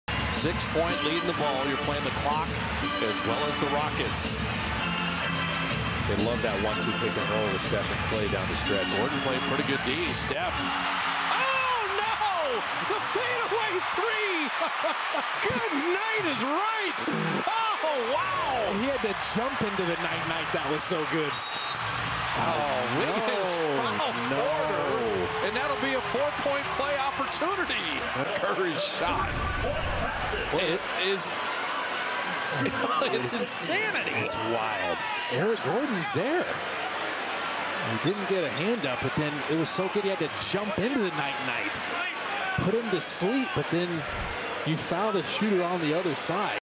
NBA Steph Curry Crazy Fadeaway Three Pointer
Curry does a insane and crazy three pointer against the rockets and does his famous celebration to add salt to the wounds!